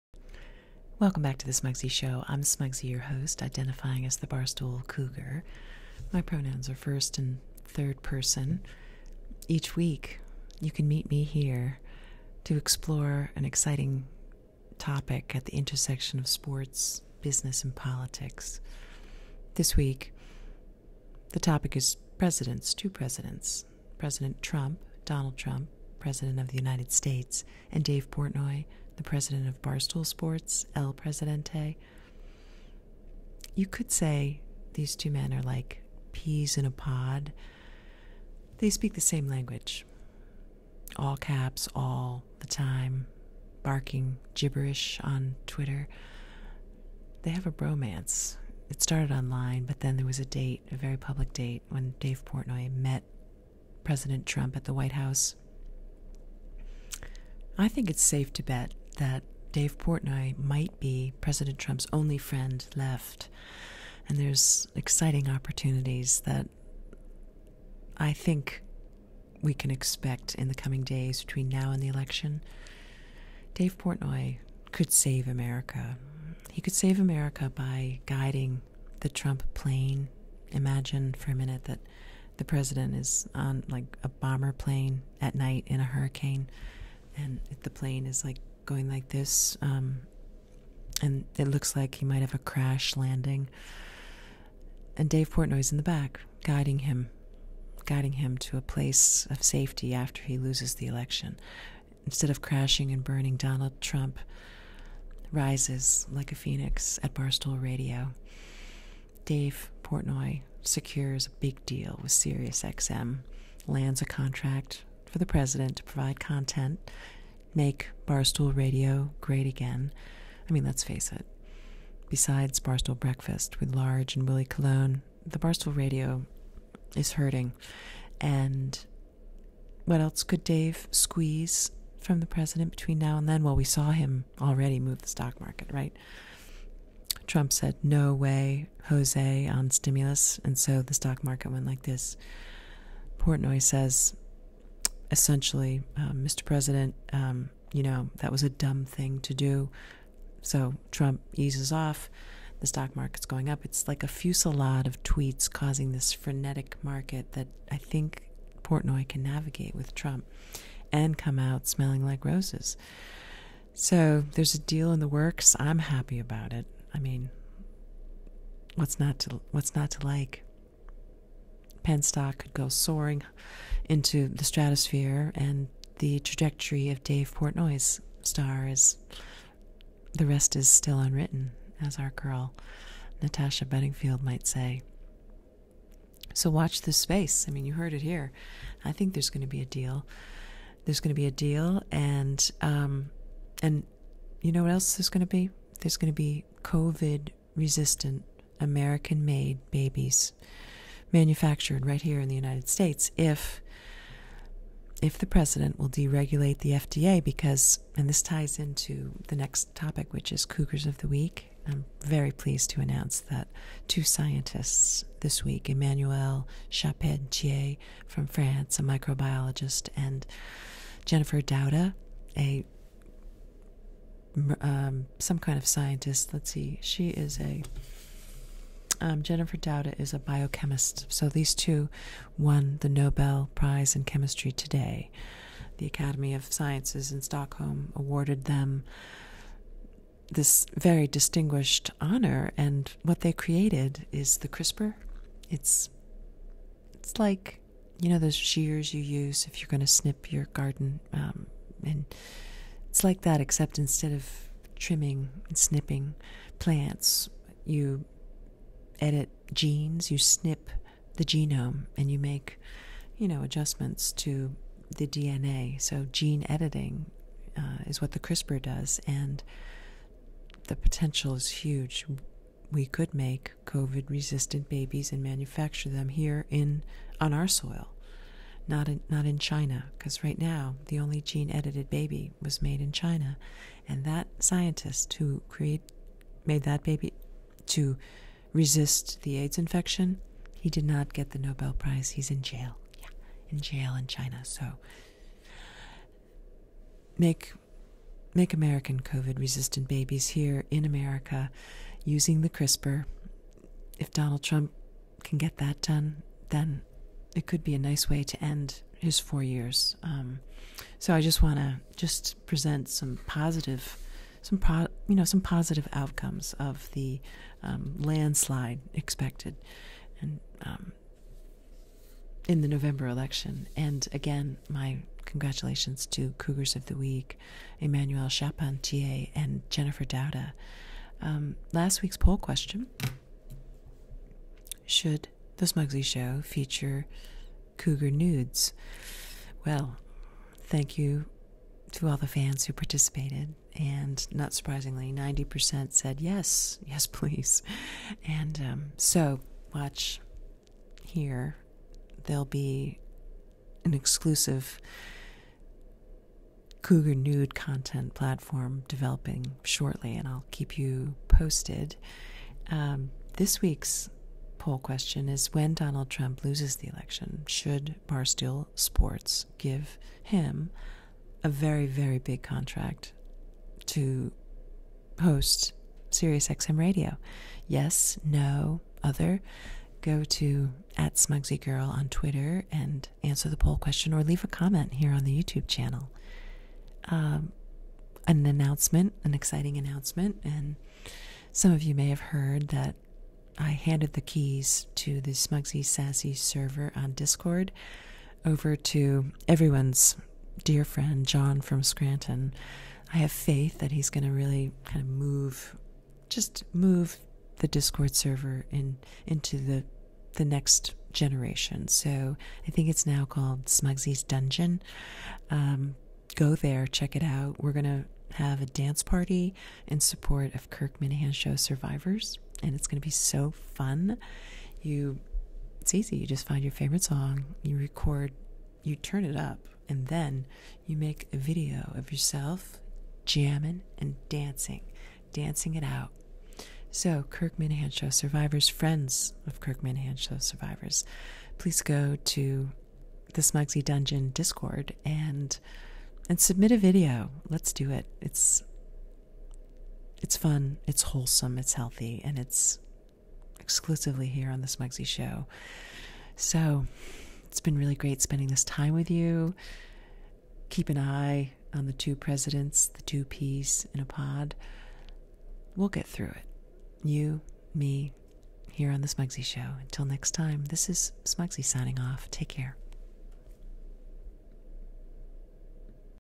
Trump to make Barstool Radio great again, Portnoy push for PENN & COVID resistant babies made in USA
This week on the Smugsy Show the bromance between the president of the US and the president of Barstool Sports is analyzed as we head into the final weeks of the campaign for president and negotiations with SiriousXM Radio, plus Cougars of the Week create the CRISPER! Don't miss it.